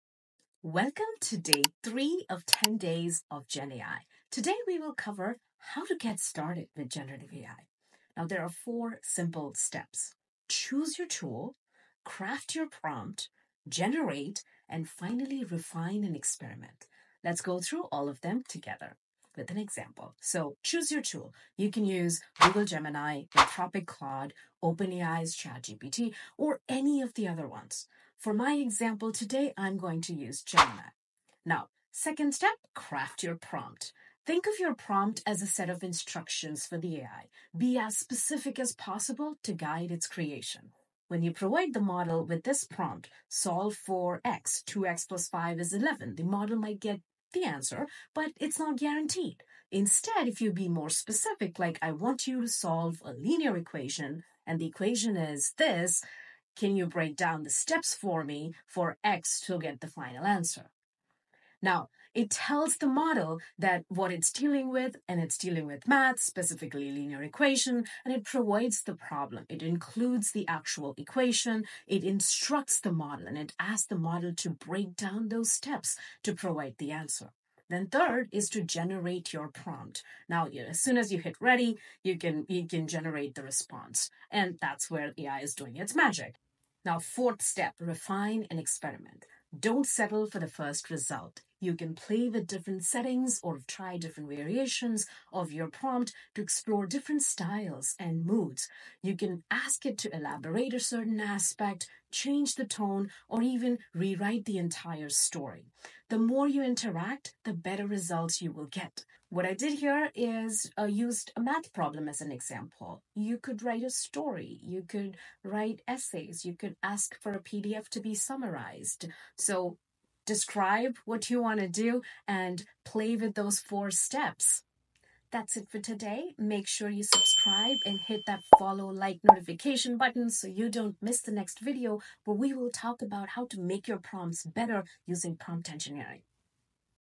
Getting started with Generative AI
10DaysOfGenAI10 Days of Gen AI: Day 3 - How to get started with Generative AI? How to get started with Gen AI?

Curious about AI that creates art, writes stories, or even codes?  This video is your friendly introduction to Generative AI! We'll break down the basics in simple terms, no tech jargon required:

What is it? Discover how Generative AI goes beyond recognizing patterns and actually creates new content.
Why is it important? Find out how this tech is already changing industries and opening up new possibilities.
How to get started: We'll explore easy-to-use tools and resources to kickstart your own AI journey.
Real-world examples: See Generative AI in action and get inspired by its amazing capabilities.
Whether you're a student, creative, or just curious about the future, this video will show you that Generative AI isn't just for tech experts – it's for everyone!